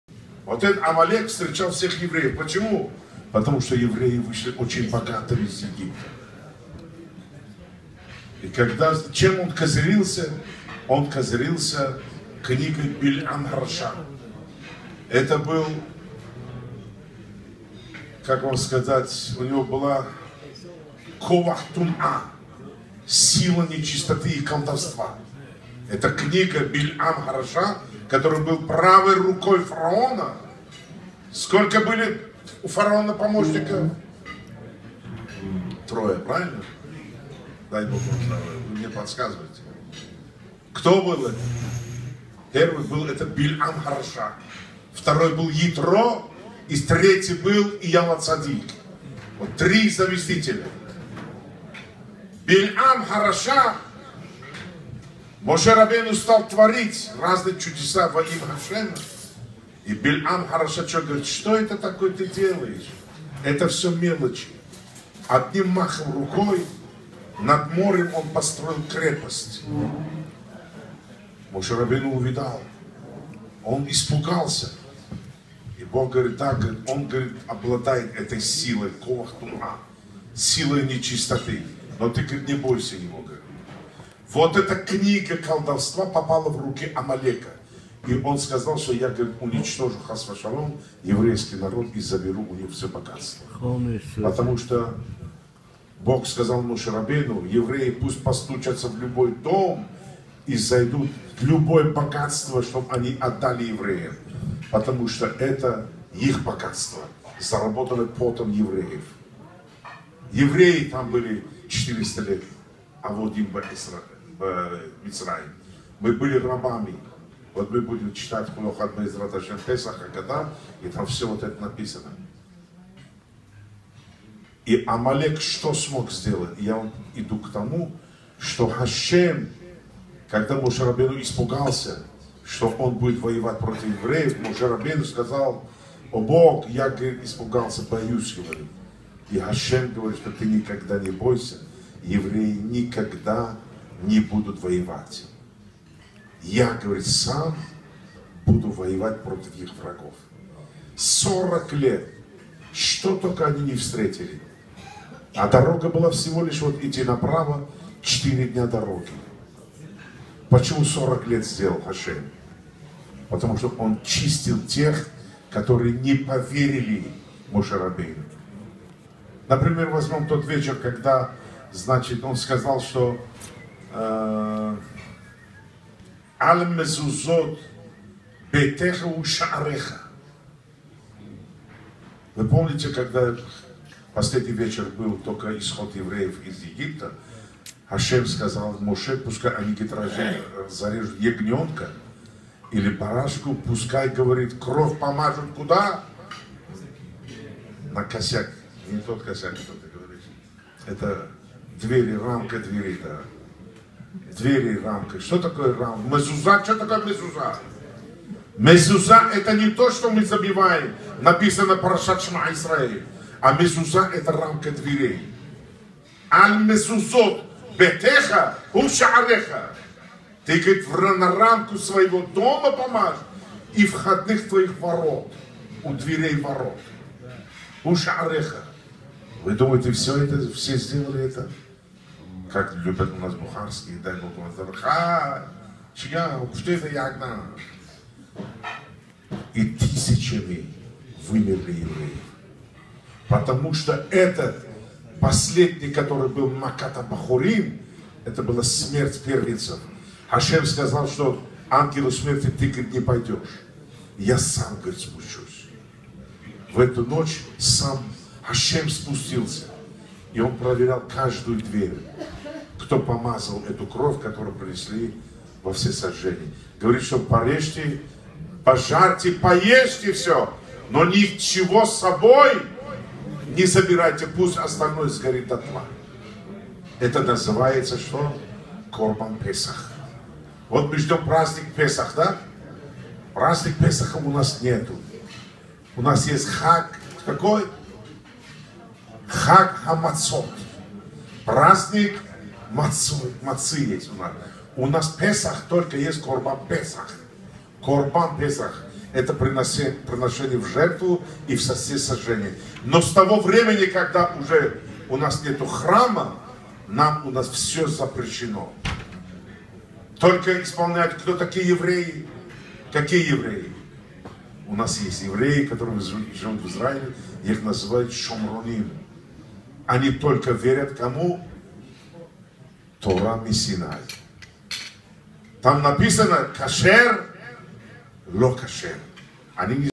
IMG 03271, Misha Normatov, ROSHEL AMINOV
ONE YEAR AZKARA-YUSHUVO  IN LOVING MEMORY OF YAKOV BADALOV BEN BURKHO AND AZARYO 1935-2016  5 ADAR 2  5776, HUSBAND OF  NINA MIRZOKANDOV, FATHER OF ILYUSHA AND MAYA, Rn "LAMOUR", QUEENS, NY 03/15/2017